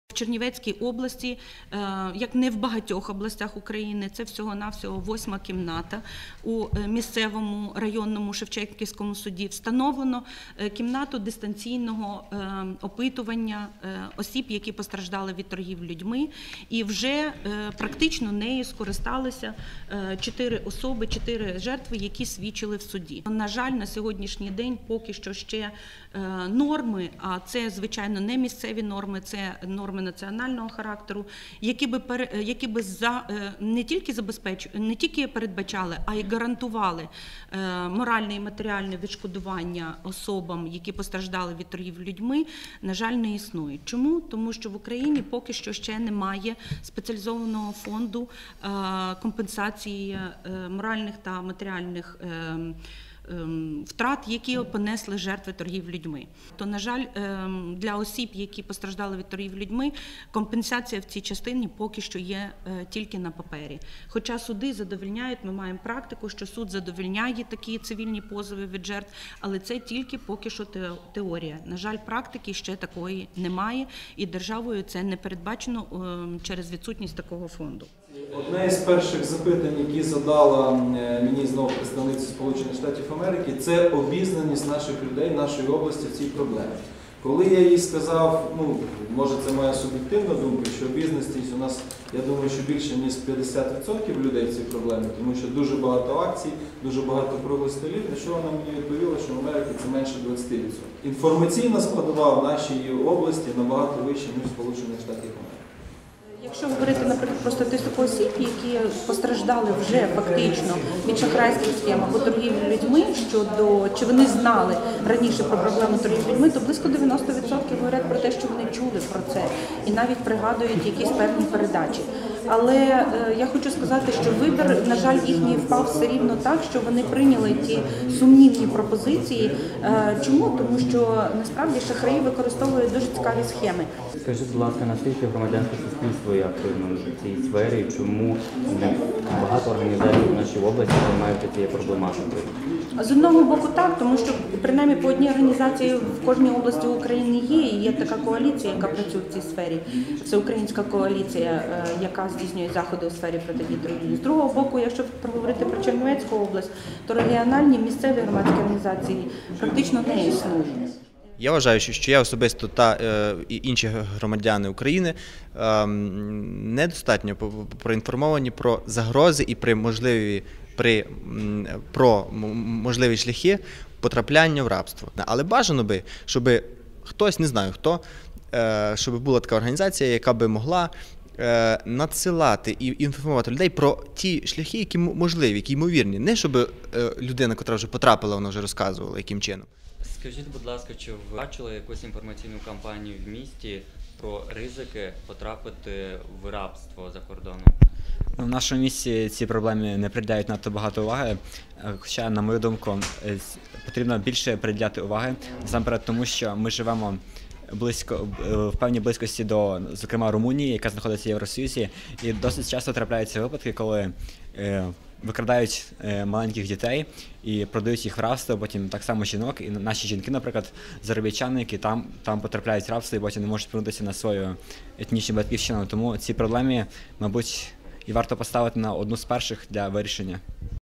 Від початку нового року на Буковині зареєстровані 4 випадки торгівлі людьми
22
березня 2013 р. в приміщенні Чернівецької обласної державної адміністрації відбувся круглий стіл з питань протидії торгівлі людьми. За словами організаторів, у 2013 р. були офіційно зареєстровані 4 звернення від постраждалих від торгівлі людьми до органів місцевої влади за отриманням статусу та комплексною допомогою. Однак, жодних неофіційних даних про фактичну кількість людей, що постраждали від торгівлі людьми не були оголошені.

Про заходи спрямовані на протидію торгівлі людьми, про рівень обізнаності наших громадян щодо загроз постраждати від торгівлі людьми та про якість проведених інформаційних кампаній у цій галузі дізнайтеся з нашого відеорепортажу.

Георгій Боднараш